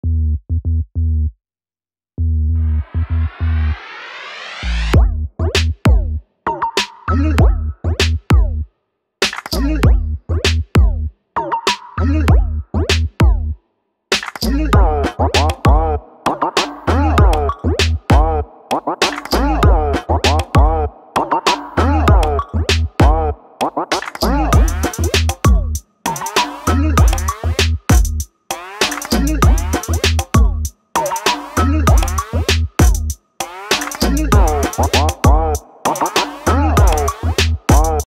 2021 Cedar Creek Champagne 38EFK Tour - Fifth Wheel by Forest River RV
Enjoy this / TikTok Tour of the 2021 Cedar Creek Champagne 38EFK. Check out the TikTok / Playlist for more!